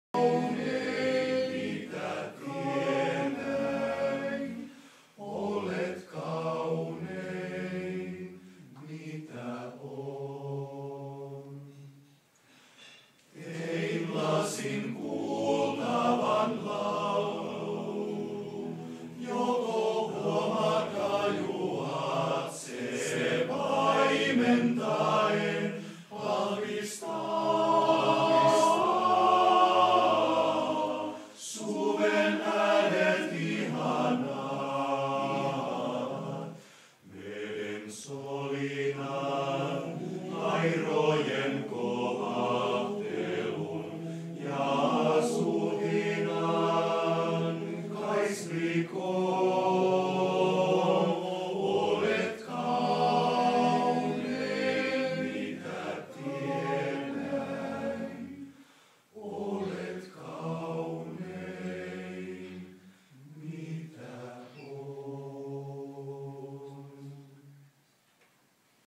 Helsinki Academic Male Choir KYL
April 7, 2010 at the residence of the Finnish Consul General, Los Angeles